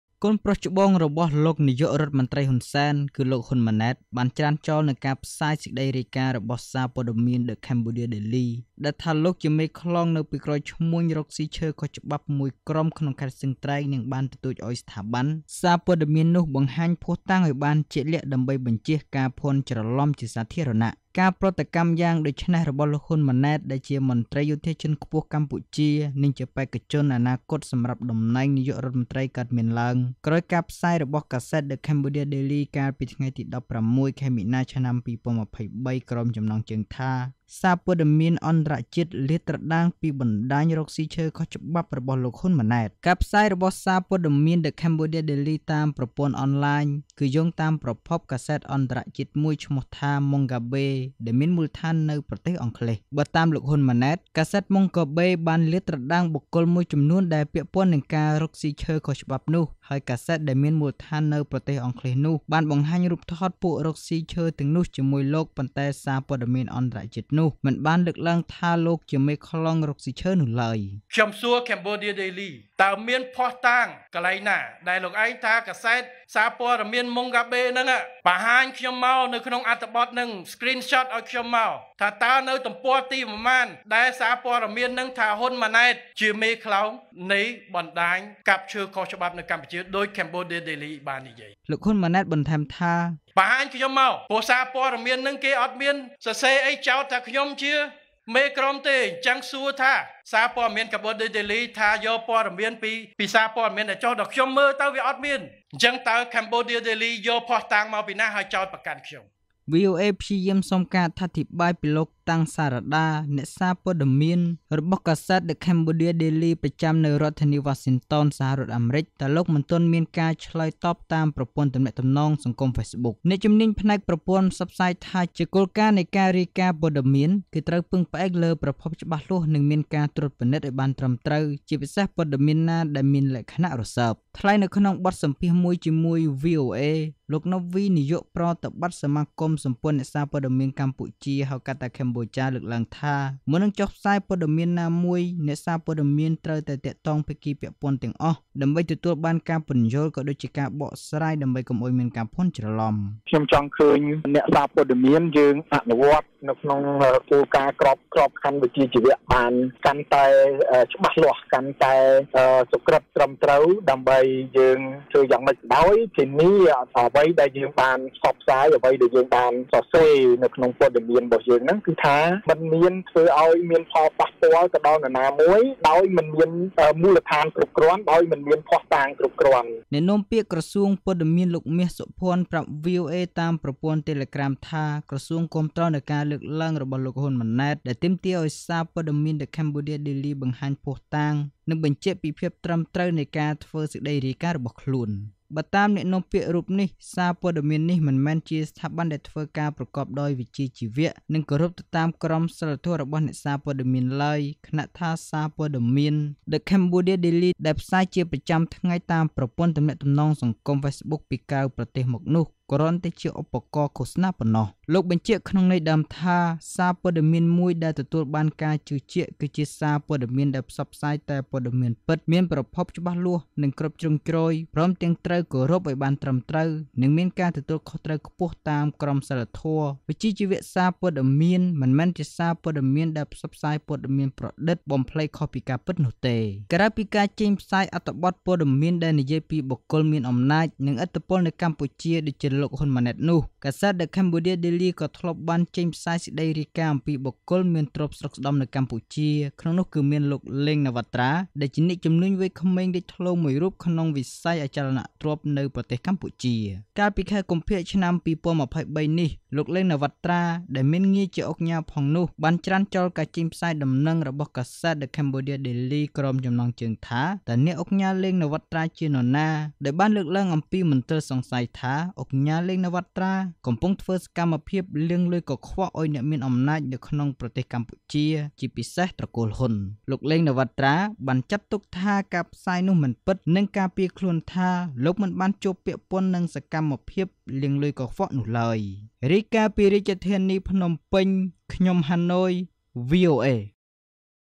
លោក ហ៊ុន ម៉ាណែត​ ច្រានចោល​ការផ្សាយ​ដែលថាលោក​ពាក់ព័ន្ធនឹង​ក្រុមរកស៊ី​ឈើខុសច្បាប់
ក្រោយ​ការផ្សាយ​របស់​សារព័ត៌មាន ​The Cambodia Daily ដែលយោង​តាម​សារ​ព័ត៌មាន​អន្តរជាតិ​មួយ​ថា ​លោក ហ៊ុន ម៉ាណែត​ បាន​ស្ថិត​ក្នុង​​បណ្តា​ញ​ឈ្មួញ​រកស៊ីឈើ​ខុស​ច្បាប់​មួយ​ក្នុងខេត្តស្ទឹង​ត្រែង​​ កូន​ប្រុសច្បង​របស់​លោក​នាយករដ្ឋ​មន្ត្រី​ ហ៊ុន សែន ​រូបនោះ​ បាន​ប្រតិកម្ម និង​​ទាម​ទារ​ឱ្យ​នឹង​សារព័ត៌​មាន​ដែលផ្សាយ​តាម​ប្រព័ន្ធ​អន​ឡាញ​មួយ​នោះ​ បង្ហាញ​ប្រភព​ឱ្យ​បាន​ច្បាស់​លាស់​។

ទស្សនា​វីដេអូ​របស់​យើង​ខ្ញុំ​តាម​ប្រធានបទ​ខាង​ក្រោម៖